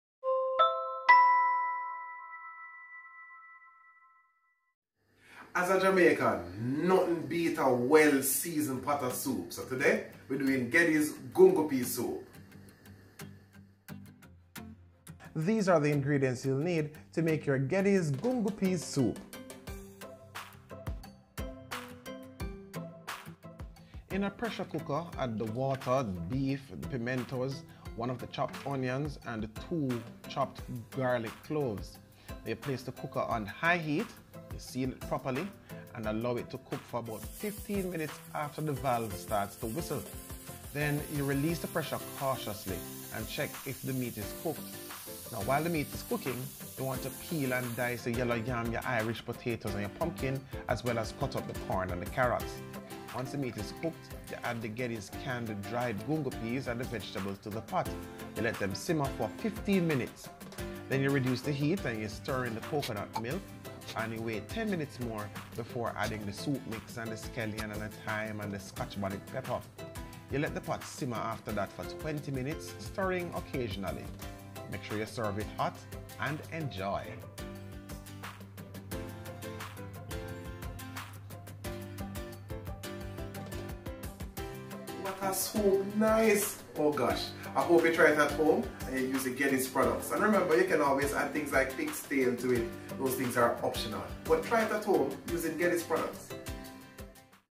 Gungo Peas Soup
Jamaicans love soup! For us, it has to be well-seasoned and preferably thick/rich/hearty. Try this Favorita Foods Gungo Peas soup then share it with your family and friends.

Follow @favoritafoods and @ohgarth on Instagram.

Favorita Foods